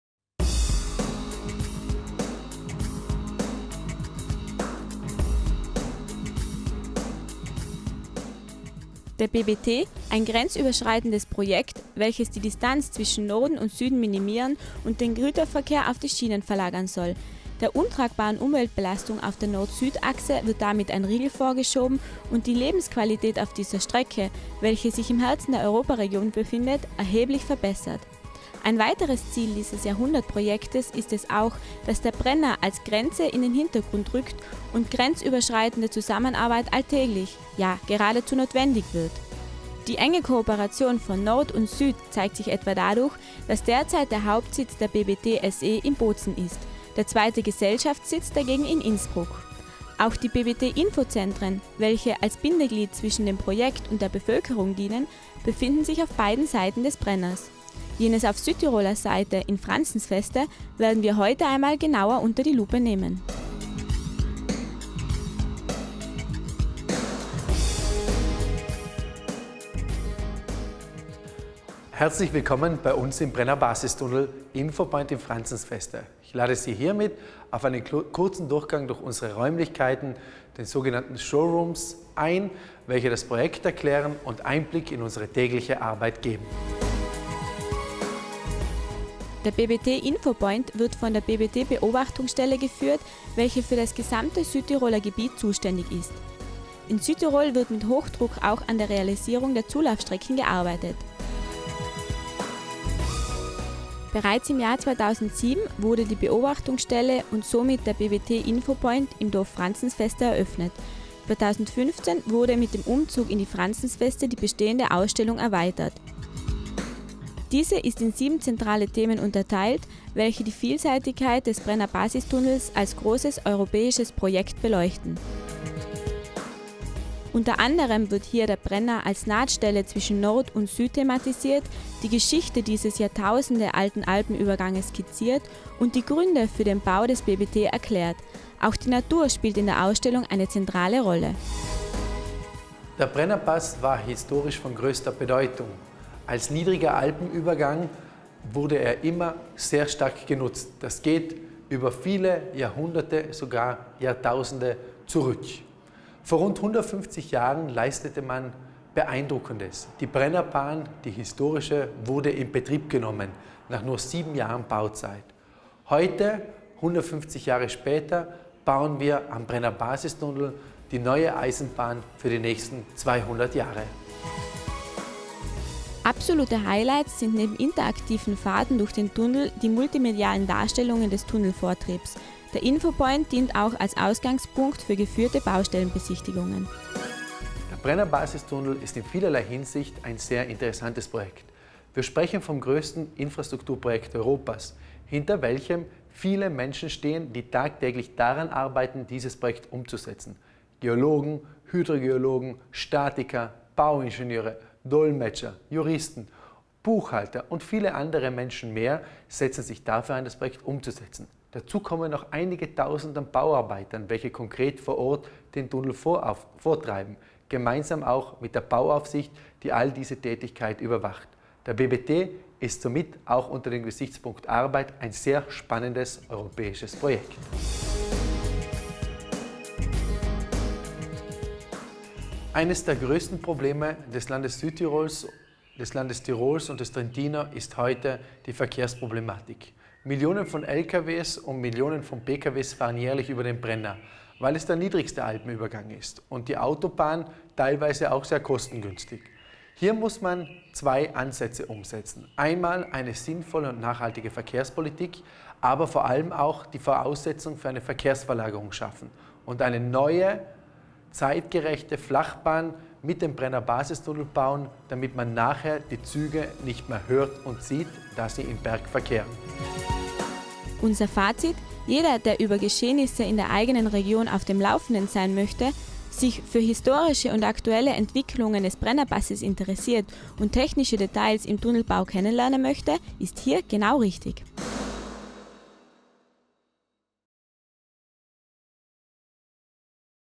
BBT-Infopoint in Franzensfeste
Im zweiten Teil unserer Videoreihe zum BBT haben wir den BBT-Infopoint in Franzensfeste besucht. Wie laufen die Bauarbeiten im Inneren des Tunnels konkret ab? Wie schauen die Geräte aus, mit denen der Tunnel vorangetrieben wird? Kann ich selbst eine Exkursion in den Tunnel machen, um die Arbeiten hautnah mitzuerleben? Diese und viele weitere Fragen werden im BBT-Infopoint in Franzensfeste beantwortet. Ein unglaubliches Erlebnis ist garantiert!